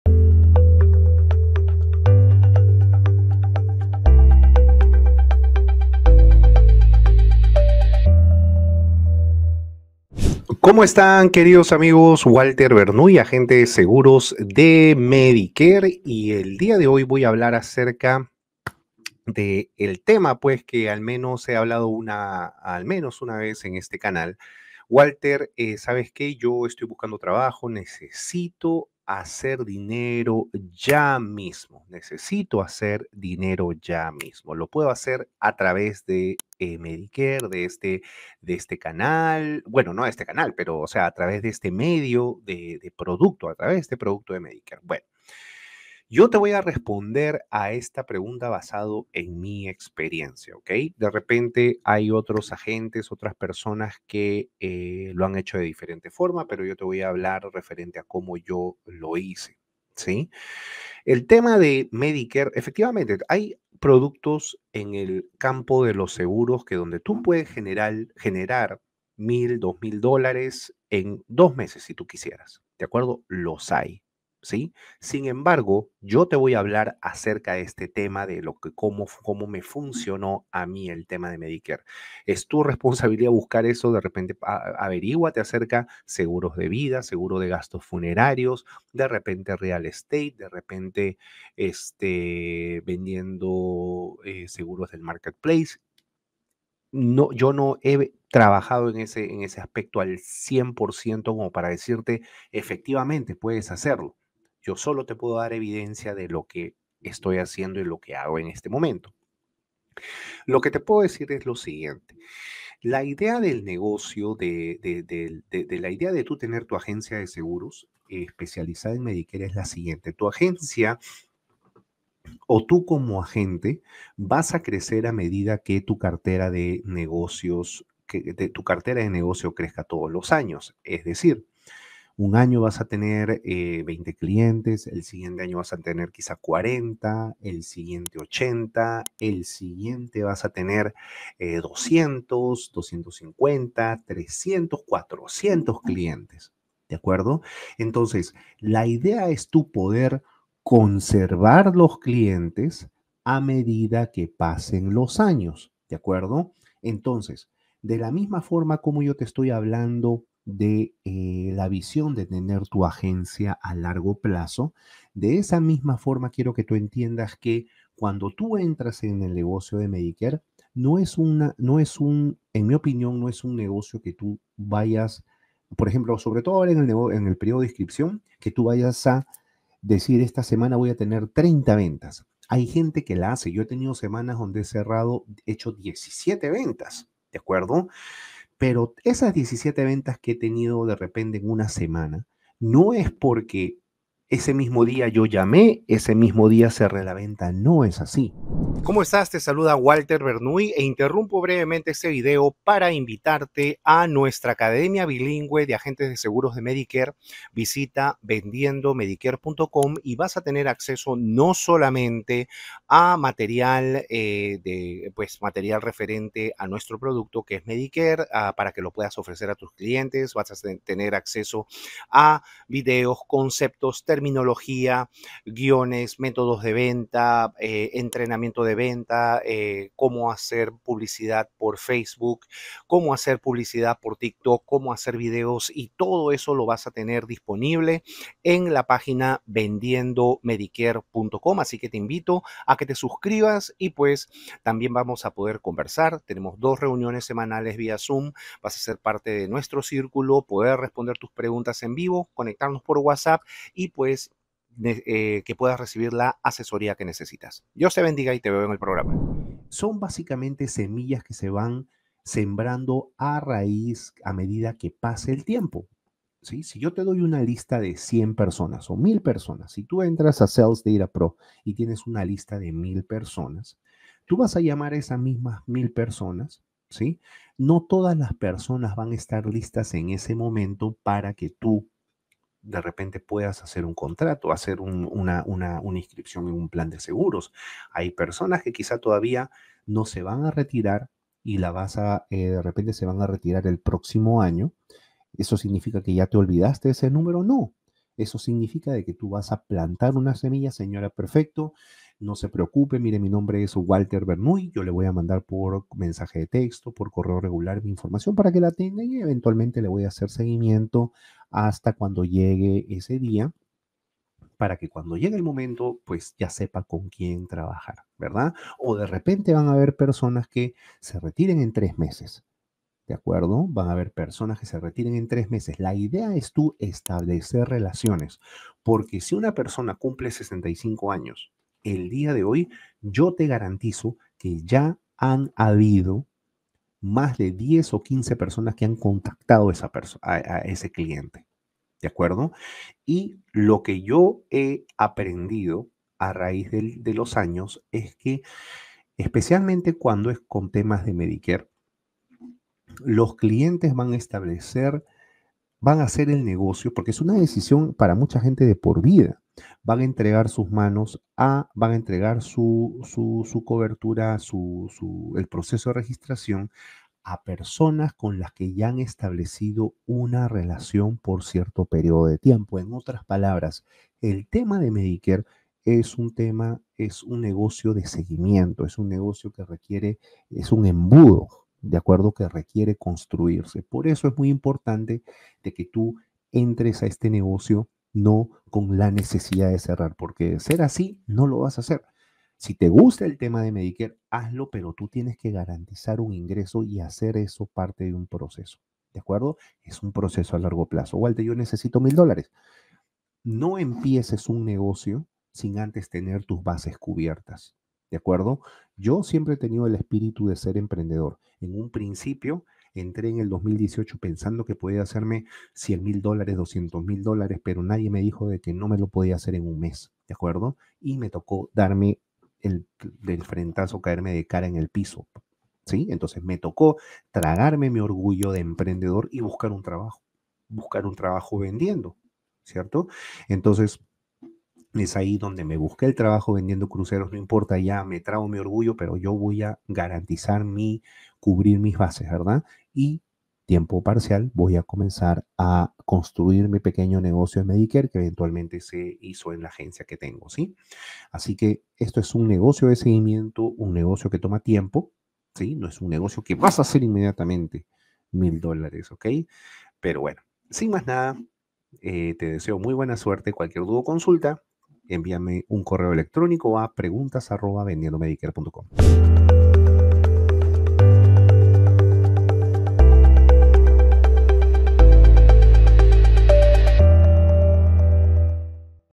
Cómo hacer dinero con Seguros de Salud | Agente de Seguros de Salud y Vida en EEUU
Walter Bernuy es Agente de Seguros por más de 10 años y dedicado a seguros de Medicare desde el 2018.  Nació en Lima Perú y llegó a los EEUU en Diciembre 9 del 2000 a la edad de 24 años. Walter Bernuy es dueño y gerente de la agencia MedSenior Savings Solutions que ayuda a clientes en más de 70 estados dentro de la Unión Americana.  Es Notario Público, Contador de profesión, autor y capacitador.